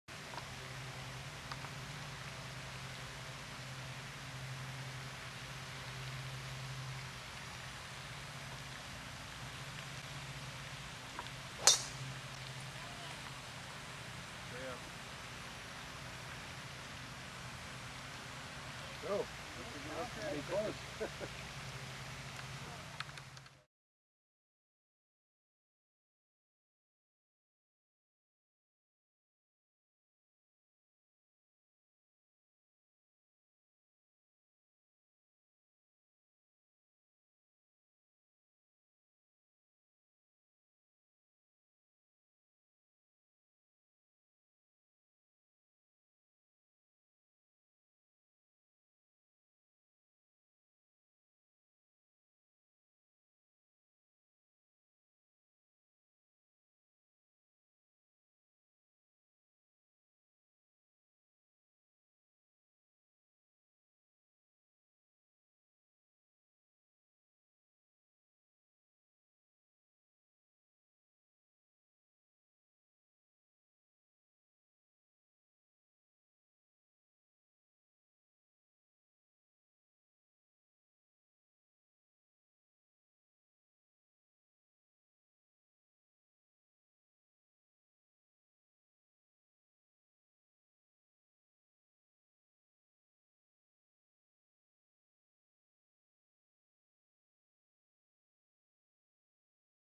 SHANSHAN FENG - DOWN THE LINE DRIVER GOLF SWING 2013 - REG & SLOW MOTION - 1080p HD
Golf swing dtl view of LPGA Tour pro Shanshan Feng during a practice round at the 2013 CME Group Titleholders tournament at Tiburon Golf Club in Naples Florida. Includes the golf swing in slow motion.